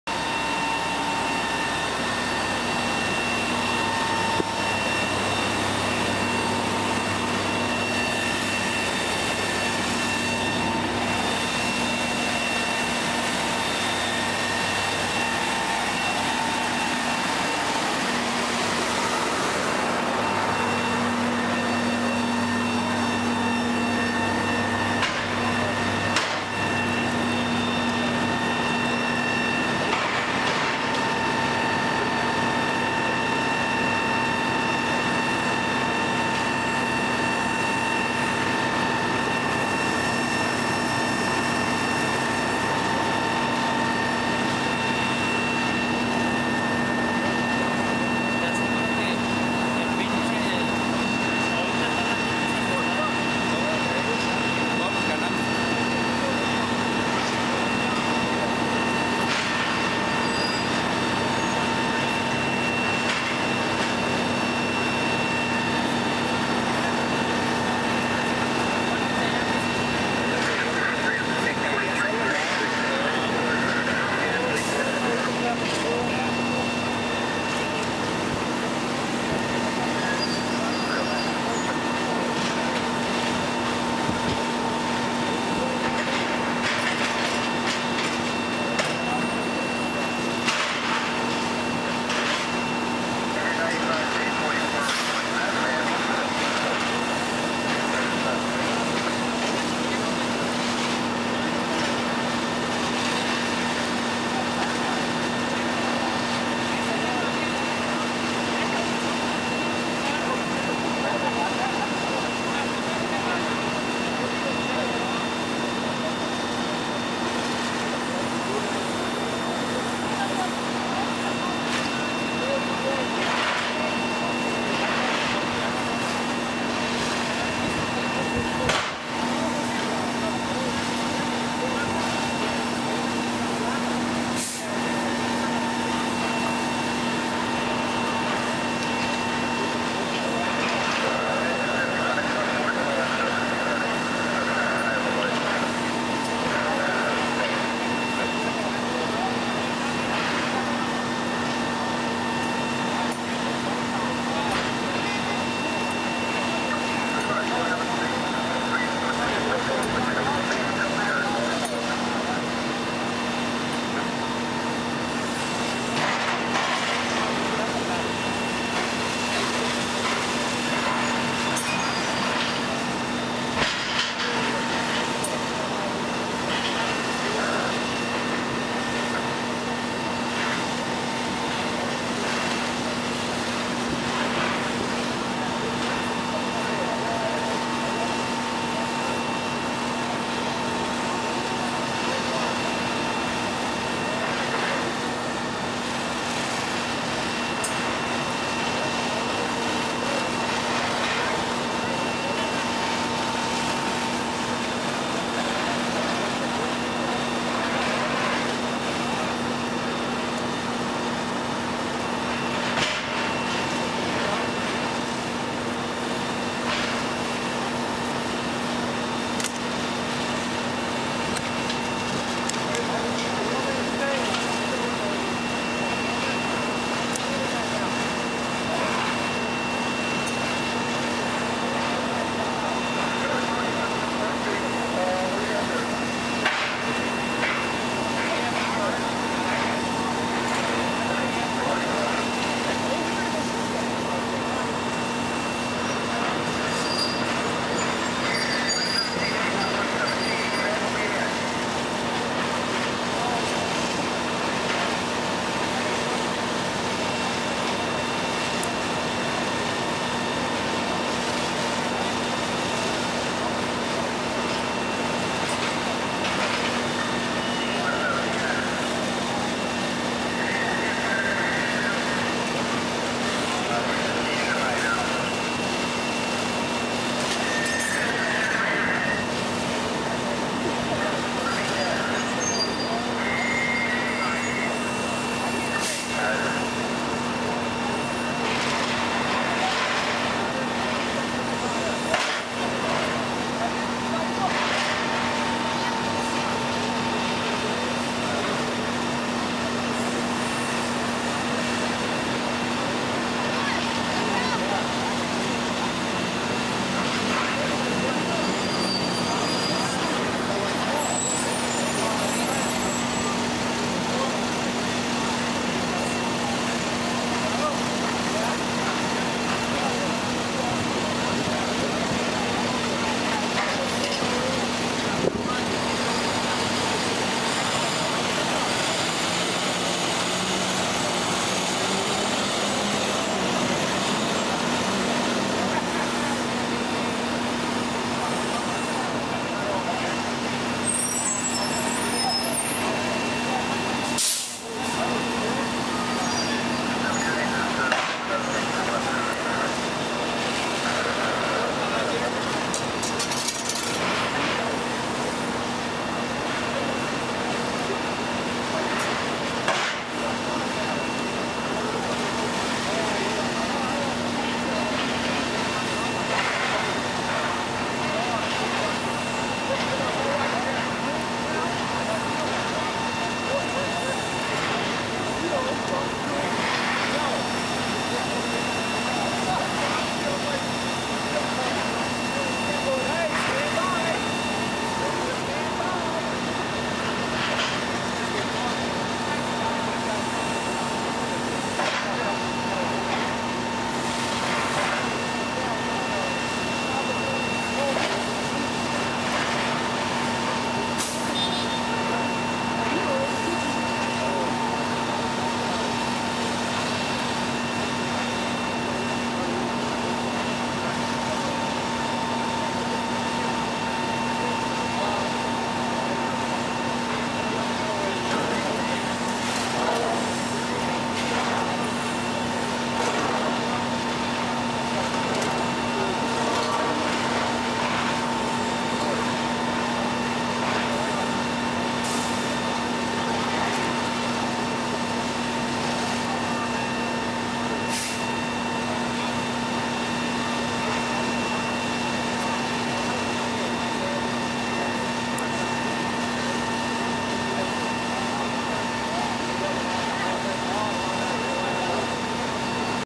Warehouse Fire Aftermath (Part 1)
Demolition Time.
CFD Tower 10, Tower 21, Tower 23, Engine 107, Engine ??, and Engine 117 (Left Early) Still On Scene.
Excavator: Liebherr 944